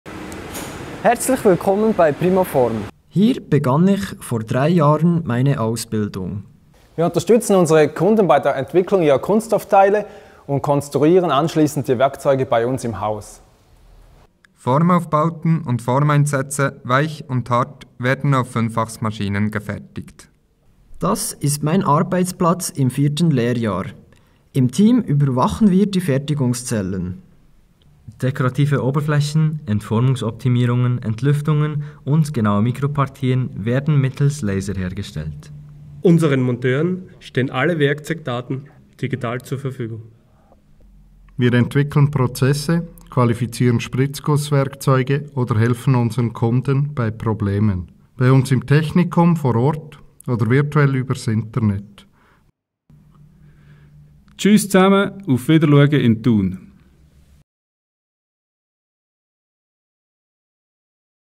Primaform AG, Werkzeug- und Formenbau, CH-Thun
Imagefilm Primaform AG, Werkzeug- und Formenbau, CH-Thun
Erstellt anlässlich des Sieges in der Kategorie extern unter 50 Mitarbeitenden im Branchenwettbewerb Excellence in Production zum Werkzeugbau des Jahres 2023.

Werkzeug- und Formenbau erfordern Präzisionsarbeit auf höchstem Niveau. Für qualitativ einwandfreie Produkte müssen Maschine, Material und Werkzeug perfekt aufeinander abgestimmt sein. Und es braucht Menschen, die sich ihrer Verantwortung bewusst sind und über Fähigkeiten verfügen, komplexe Prozesse zu beherrschen.